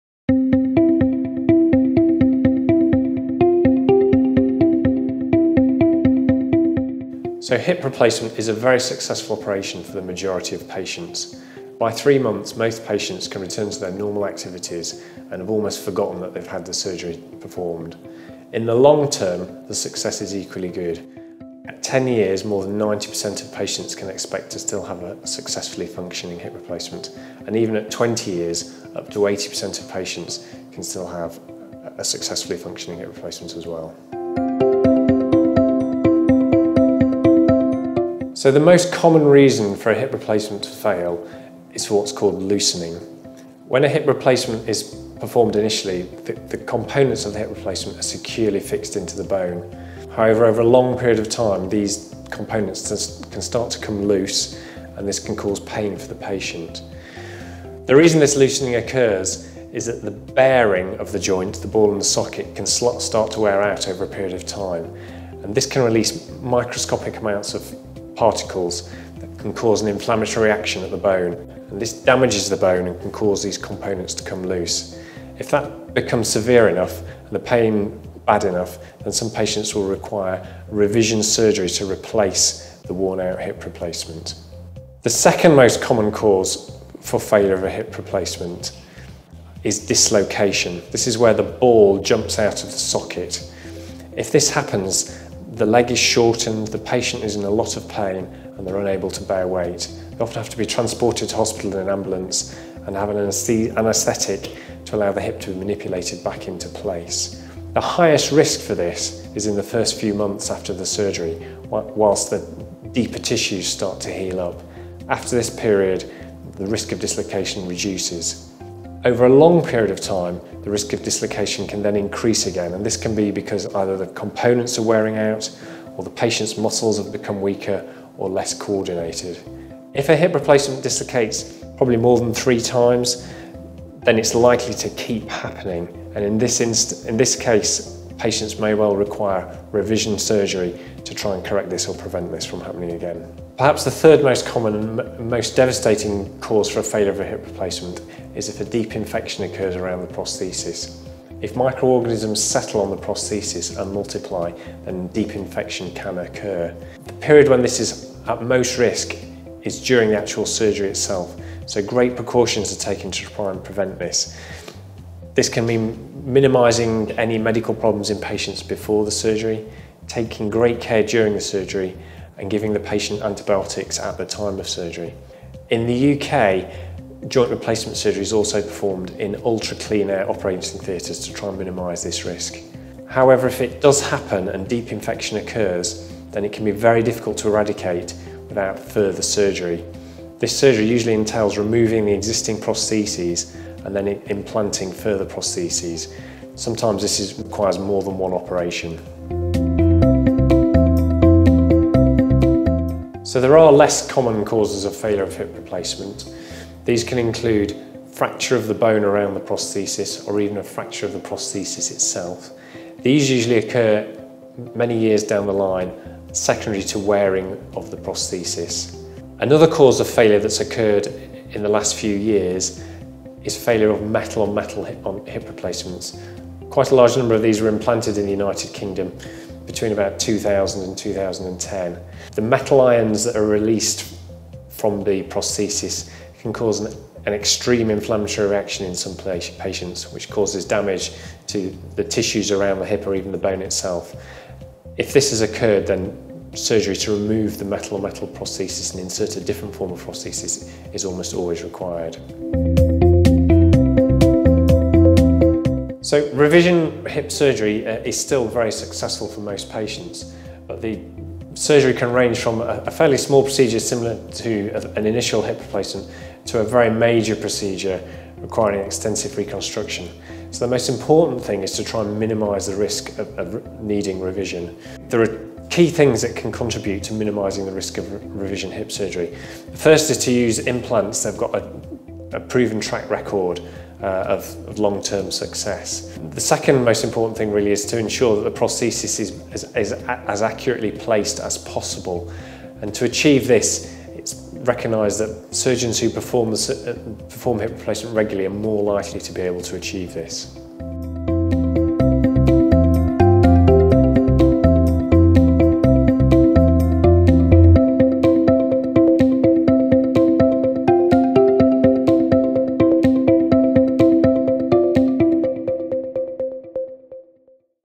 How successful are hip replacements?
Orthopaedic surgeon Mr Nick de Roeck talks about how successful hip replacements are, with 90% of patients still having a perfectly functioning hip ten years after their operation.

Learn more about hip replacements, what might cause them to fail and how to reduce the risk of failure.